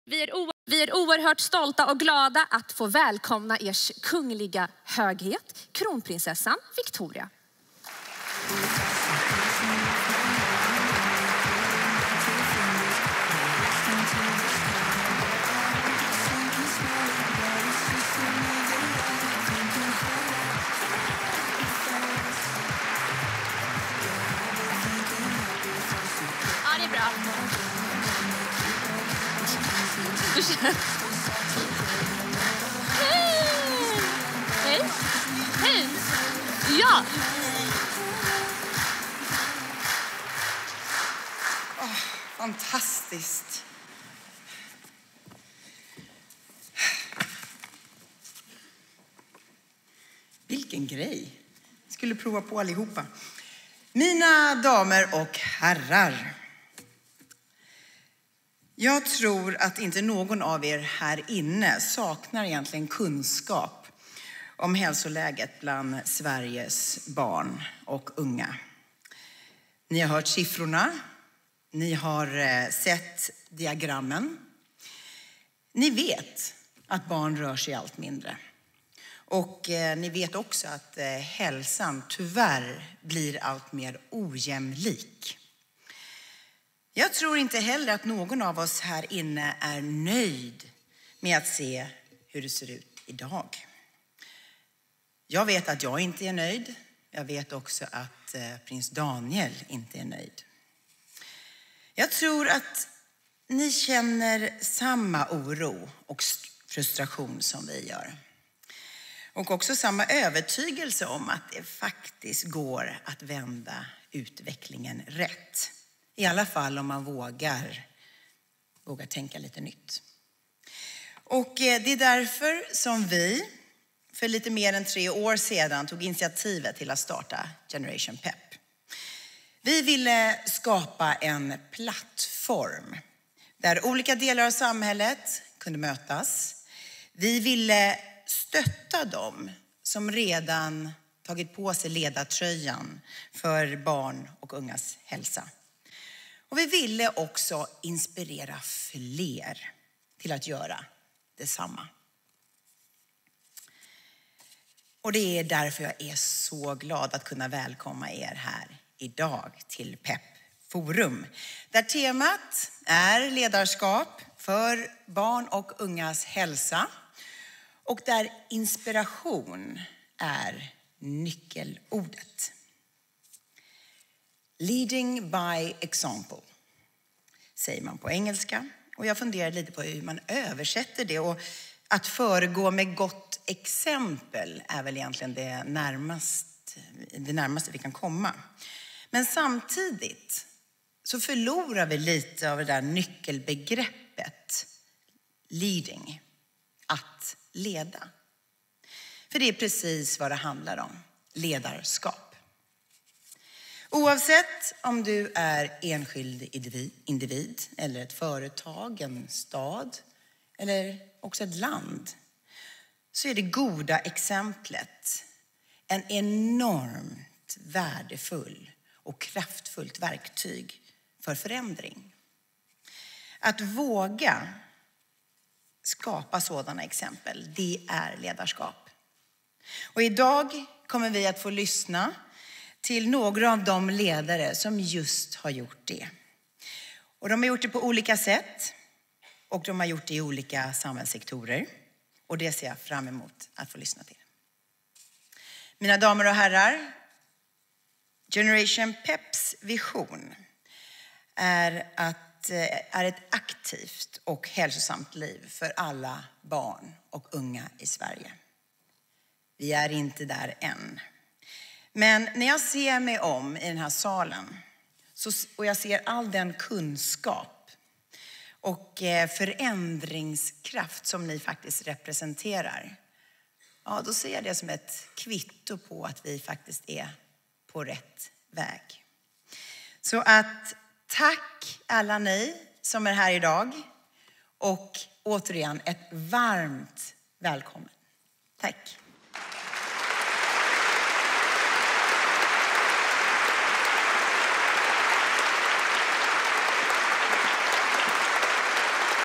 H.K.H Kronprinsessan Victoria på Pep Forum 2019
H.K.H Kronprinsessan Victoria talar på Pep Forum 2019